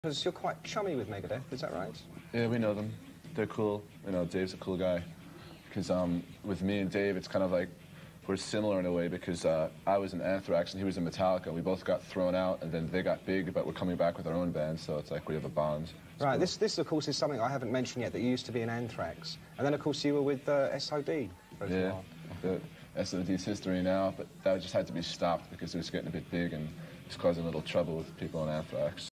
NUCLEAR ASSAULT's Dan Lilker's bond with MEGADETH's Dave Mustaine/METALLICA/ANTHRAX/S.O.D./Plague
Dan Lilker of Nuclear Assault talks about his bond with Megadeth's Dave Mustaine.